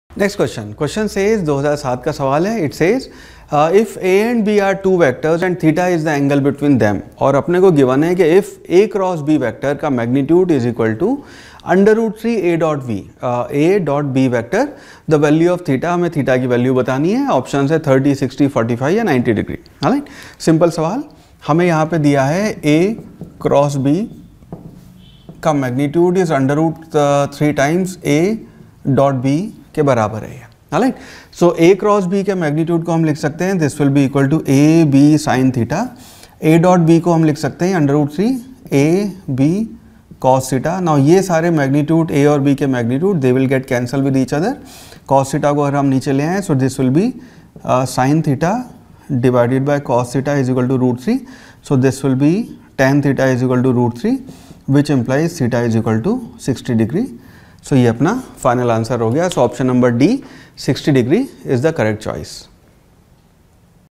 A and B are two vectors and is the angle between them, if A x B=3(A.B), the value of is (a) 45 (b)
A and B are two vectors and is the angle between them, if A x B=3(A.B), the value of is (a) 45 (b) 30 (c) 90 (d) 60